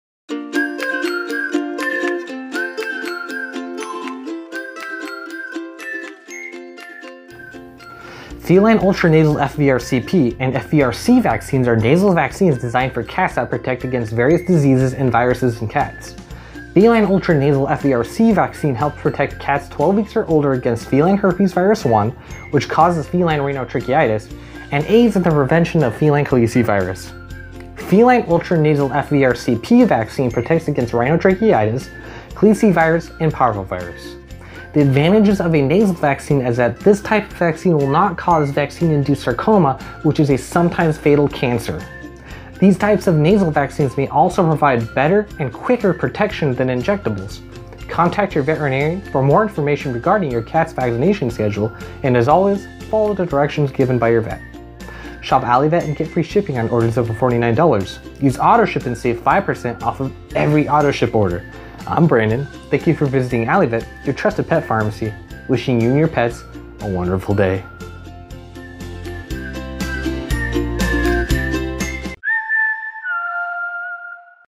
Feline UltraNasal FVRCP Vaccine
Feline Ultranasal Vaccines FVRCP and FVRC protect against many common and contagious viruses and diseases.  Using modified live viruses to prep your cat’s immune system for the real disease Feline Ultranasal Vaccines protect your feline from diseases such as rhinotracheitis, calicivirus, parvovirus and many others depending on the vaccine.  Contact your veterinarian for more information regarding your pet’s vaccination schedule and as always, follow the directions given by your vet.